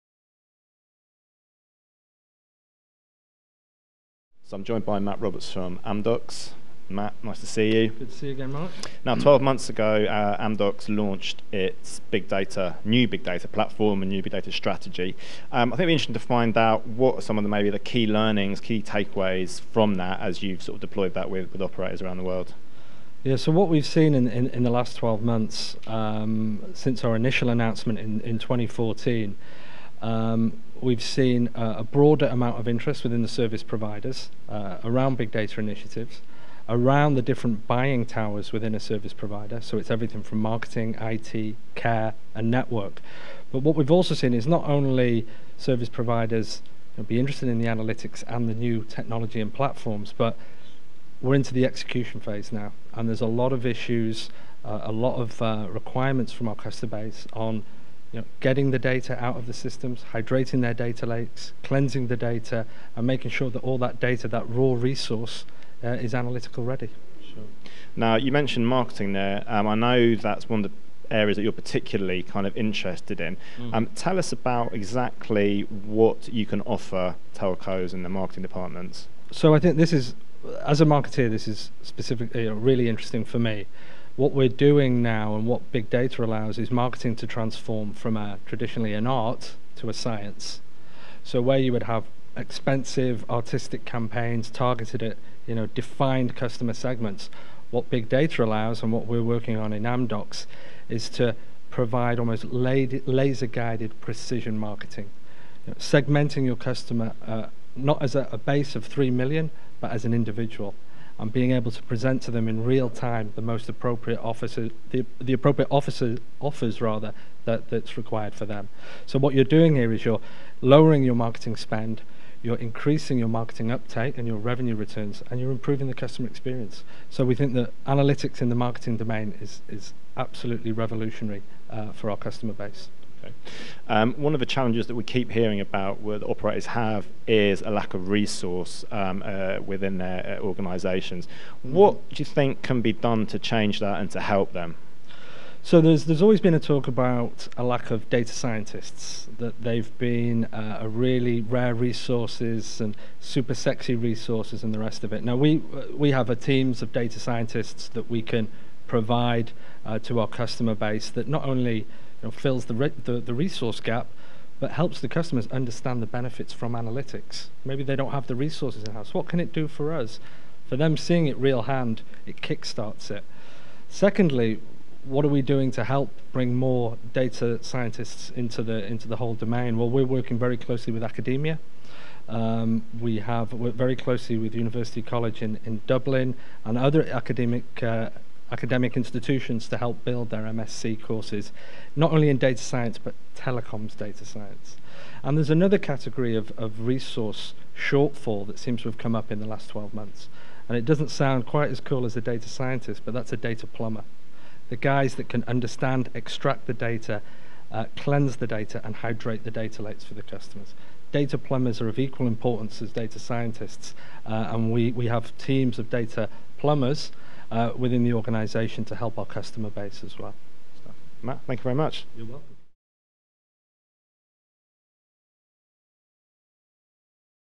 Big data seminar 2015: Sponsored Q&A with Amdocs' Matt Roberts
Amdocs Director of Big Data Marketing Matt Roberts in conversation with European Communications Editor Marc Smith